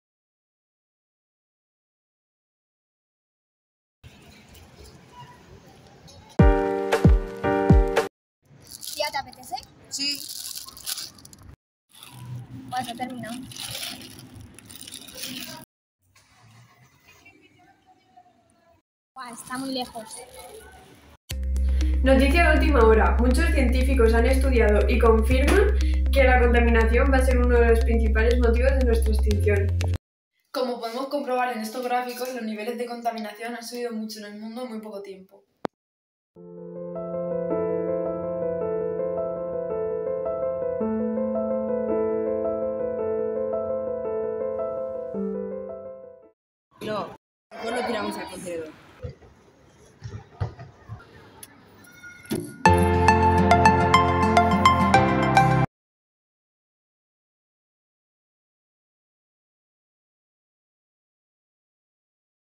EcoConciencia
Científicos apuntan a que la contaminación será el principal motivo de la extinción humana

Colegio Santo Ángel de la Guarda
Festival de Clipmetrajes de Manos Unidas